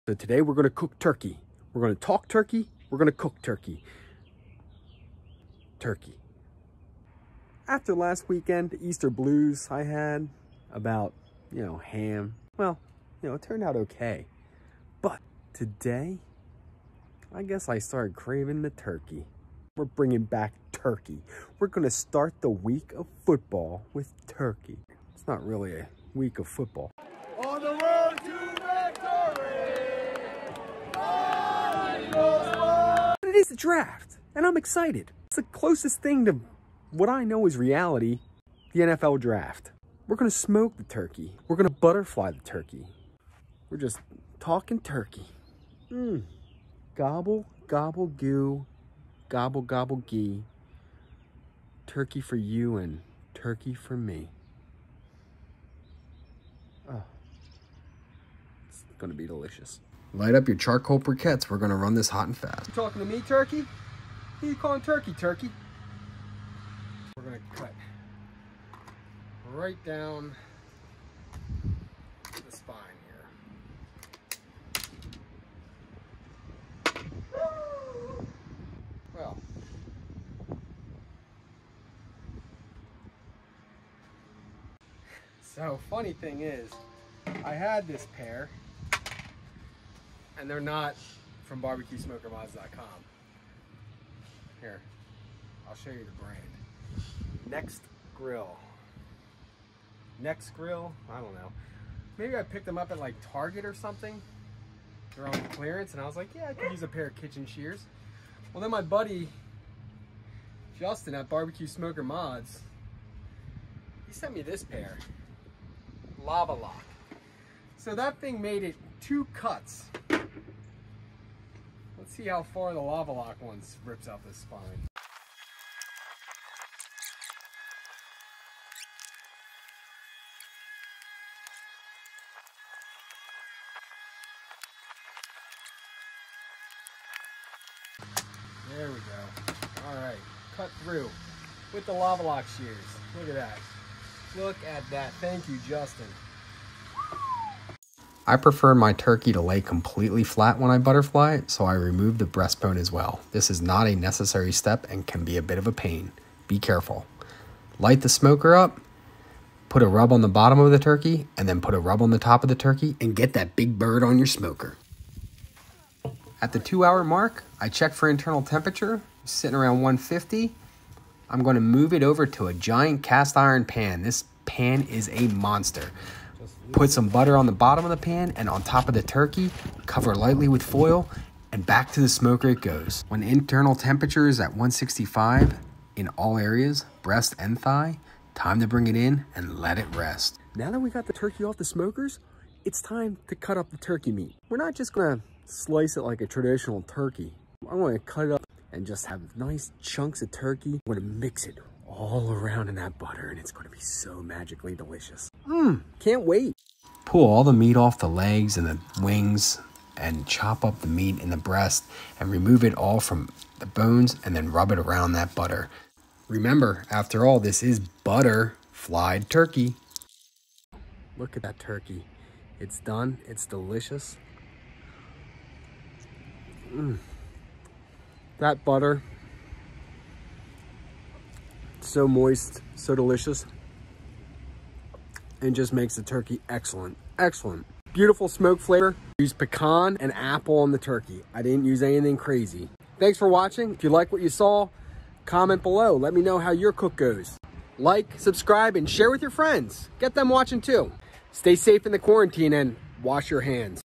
Quarantine Day 39 “Butter”flied Smoked Turkey
Let’s smoke some turkey! This video highlights more of the process of smoking a turkey as well as my general excitement around smoking a turkey. The process is simple and can be complex if you want crispier skin. I was testing out butterflying the turkey as well as providing a butter bath during the cook. stayhome and save lives

Smoker woods used 1:1 mix of pecan and apple wood.